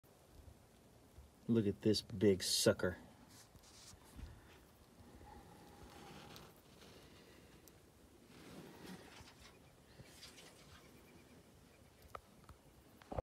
BIG RATTLESNAKE
I know some people don’t like to kill snakes but when we have little ones that like to play outside I didn’t need this sucker hanging out in the front yard!!